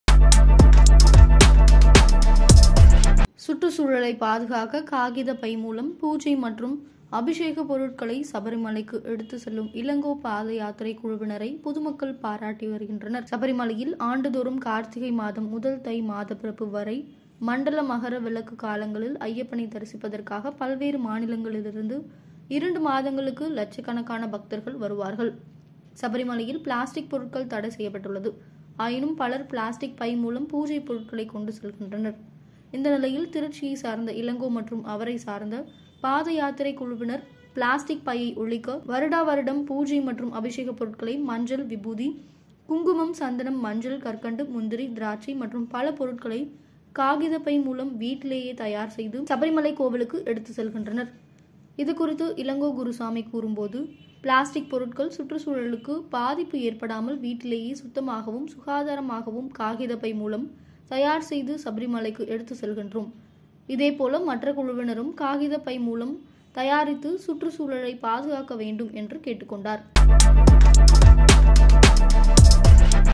சபரிமலைக்கு காகித பை மூலம் பூஜை பொருட்களை எடுத்து செல்லும் பதா யாத்திரை குழு - JMTV
சுற்றுச் சூழலை பாதுகாக்க காகித பை மூலம் பூஜை மற்றும் அபிஷேக பொருட்களை சபரிமலைக்கு எடுத்து செல்லும் இளங்கோ பதா யாத்திரை குழுவினர் - பொதுமக்கள் பாராட்டு